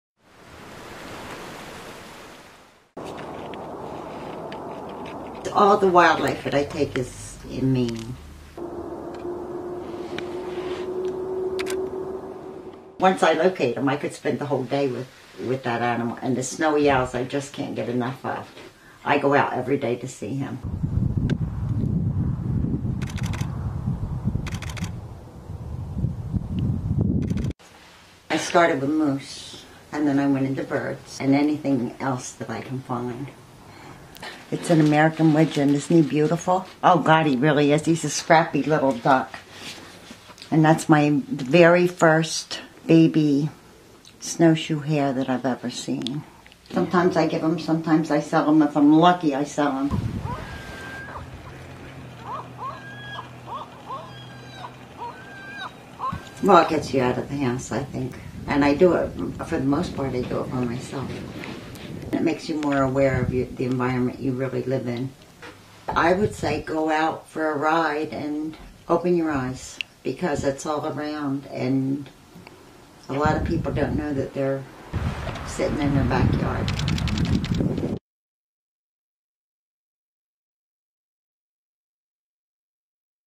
Bangor photographer brings light to local wildlife
Sharon Fiedler  64  of Bangor  has been photographing local wildlife for the past seven years. As she drives around the greater Bangor area  she talks with people she meets along the way to learn about wildlife and where she might find it. From resident bald eagles to fox families to black bears  she’s seen it all  proving that you don’t have to go deep into the Maine woods to find some of the state’s most majestic critters.